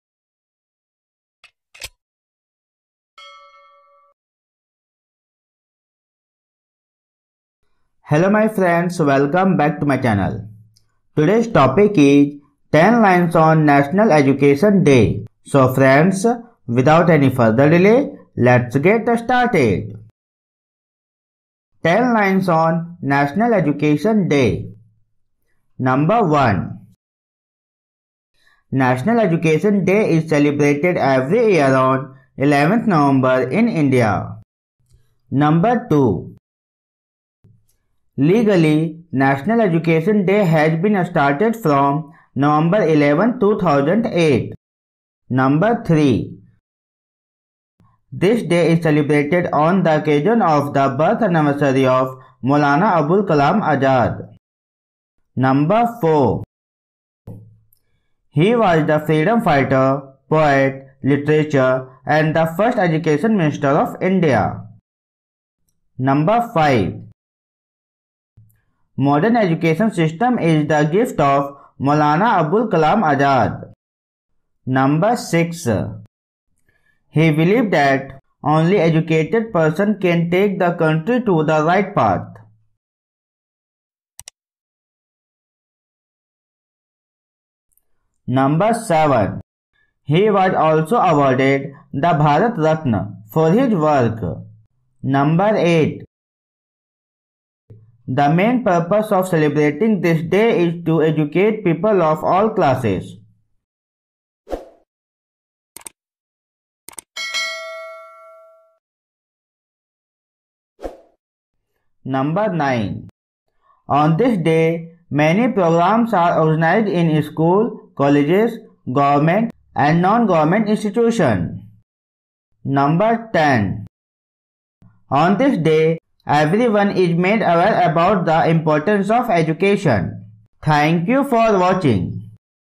10 Lines on National Education Day In English | Few Lines About National Education Day 2021
👇Visit Website & Pdf

👉 About Video : - Hello my friends today we will learn that How To Write 10 Lines on National Education Day In English | Few Lines About 10 Lines About National Education Day In English 2021